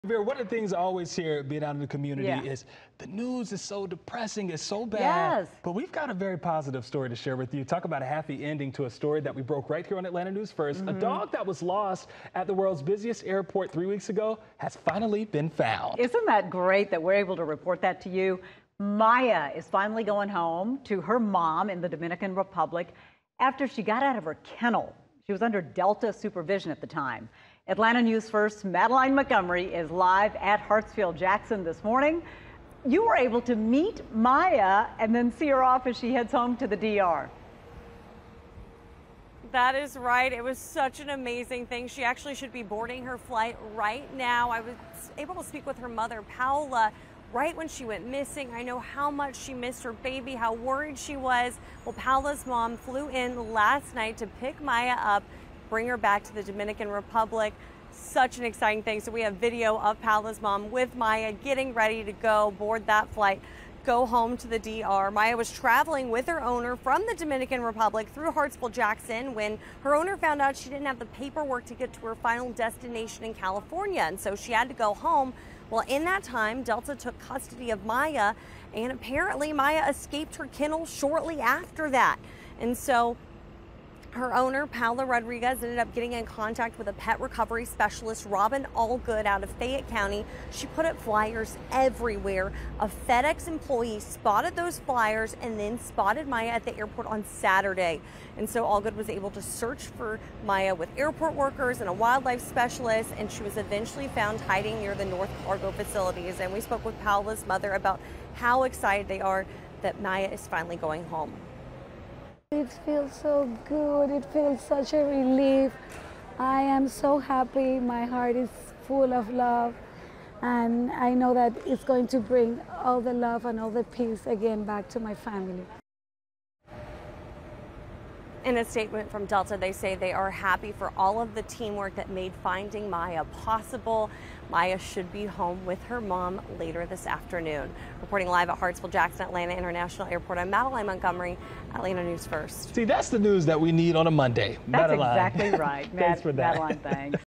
Dog lost at Atlanta airport picked up, finally flying home to reunite with owner
Maia, the dog who escaped onto the airfield at the world’s busiest airport, was picked up Monday morning by her owner’s mother and is finally headed home!